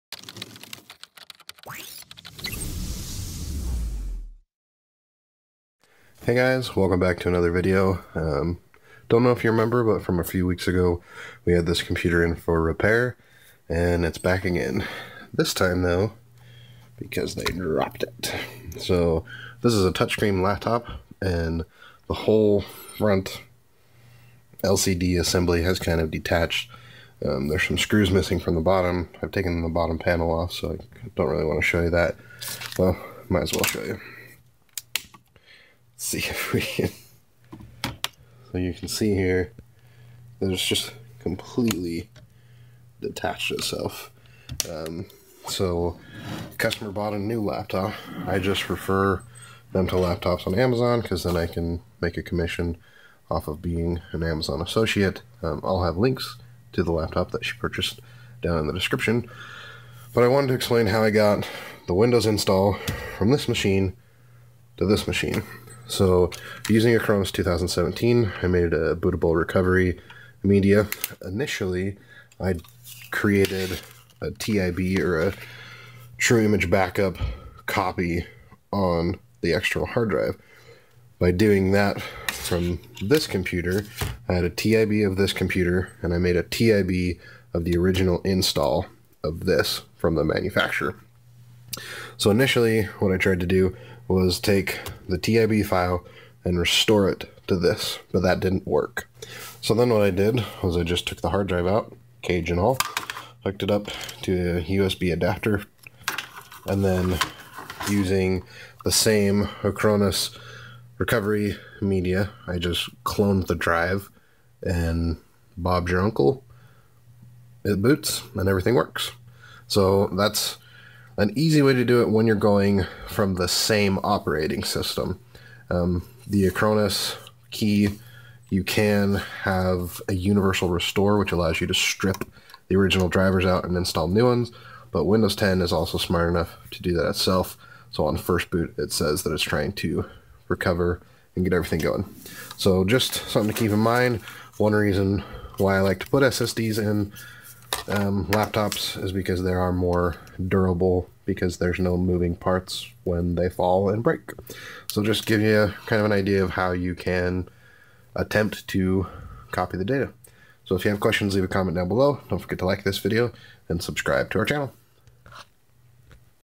Move Microsoft Windows install to a new computer
In this video, we show you how to move a Microsoft Windows install to a new computer.

Dell Inspiron 5000 Laptop

Acronis 2017

External Backup Hard Drive

Sandisk SSD

Hard drive adapter

If you have any computer questions I'd love to answer them. Ask on trenchtech.co or in the comments.